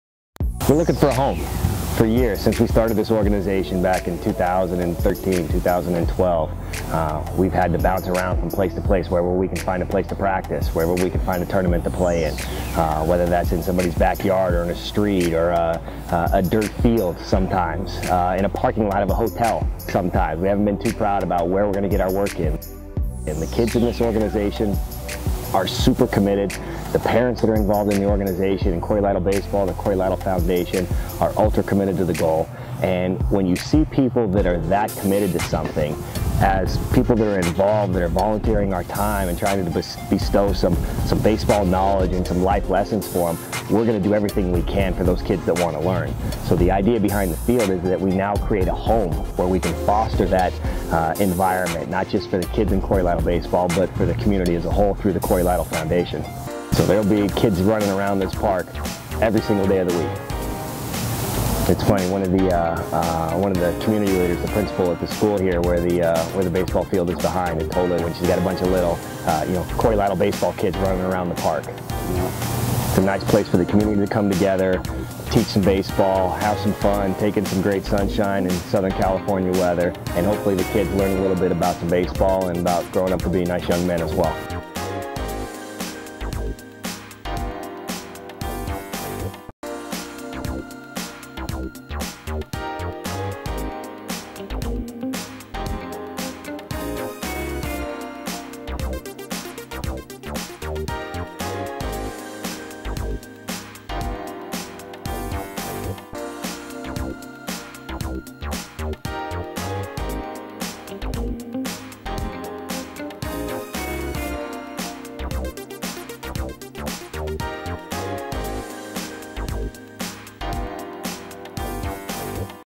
Cory Lidle Baseball coach Frank Sarver talks about
coach Frank Sarver talks about the Cory Lidle Baseball Field